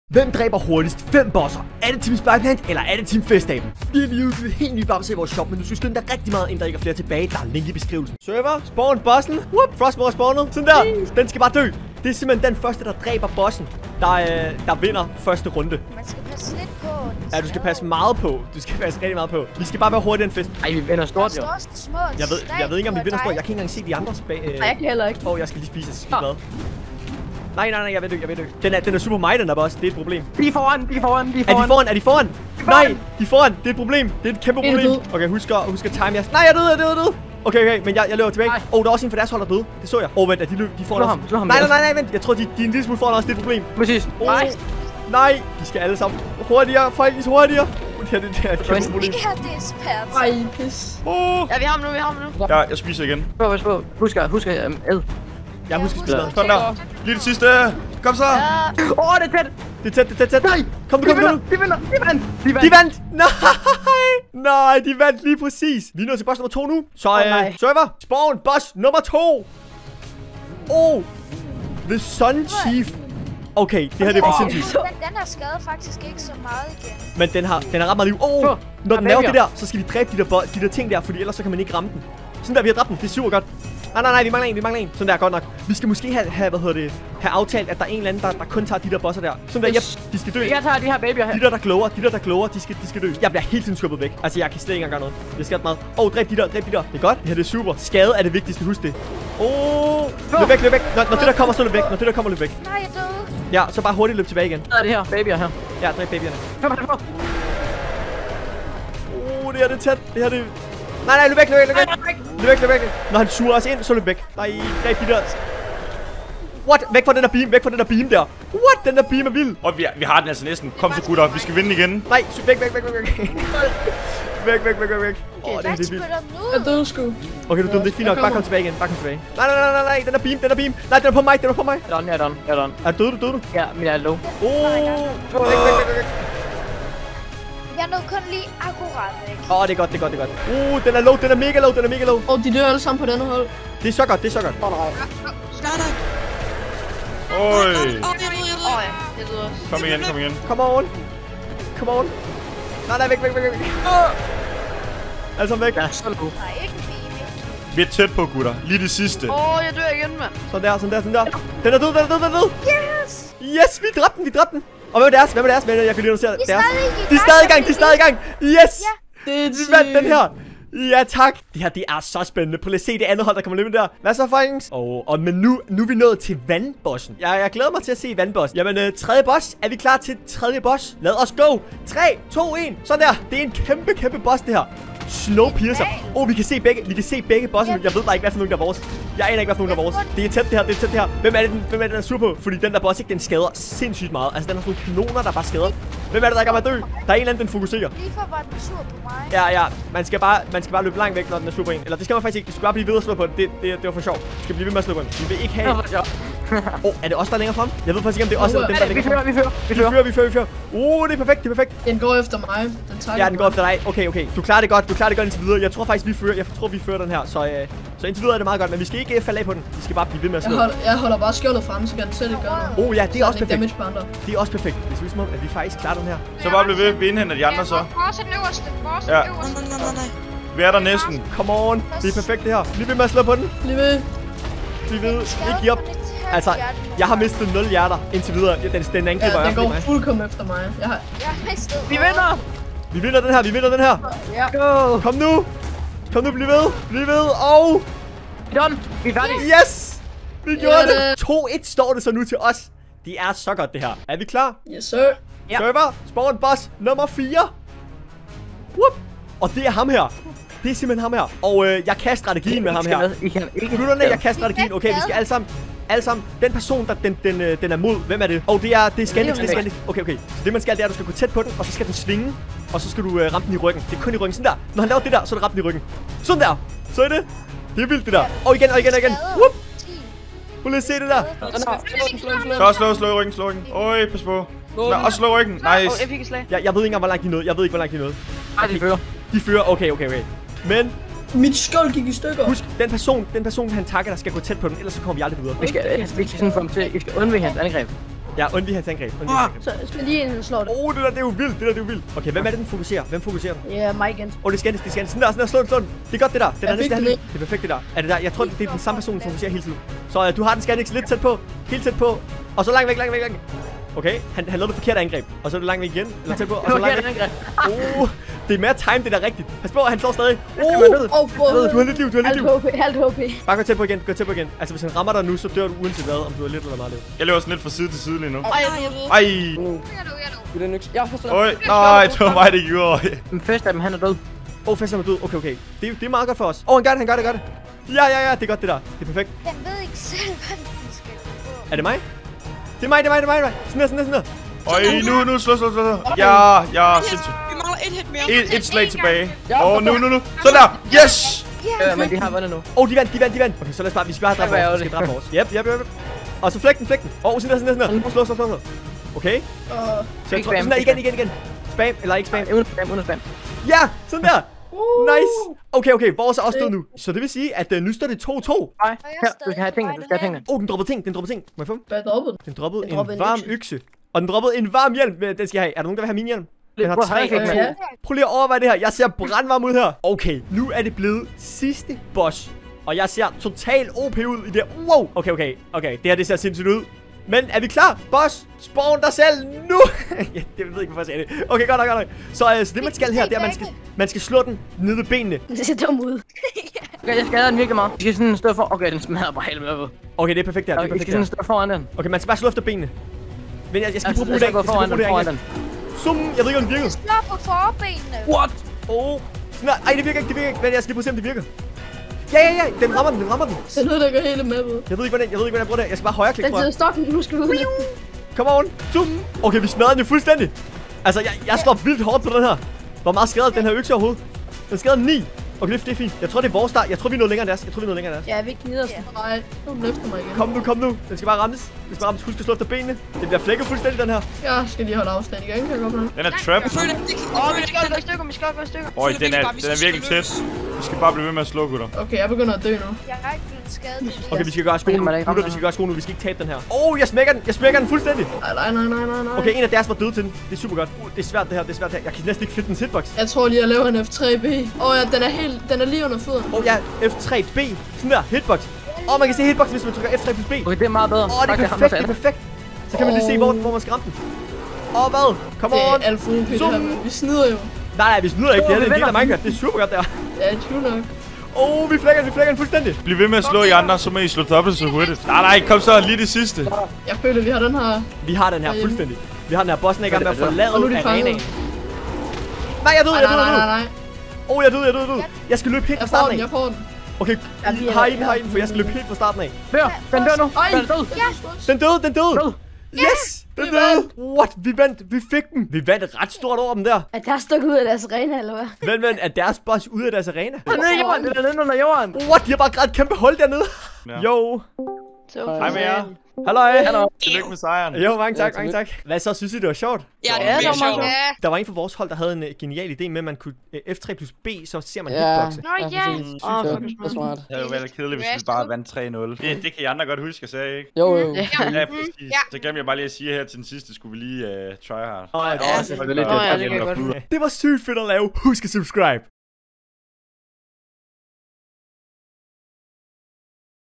10 Fans Mod BOSSER!
I dag spiller Team Spyplant mod Team @Festaben  om at dræbe bosser hurtigst!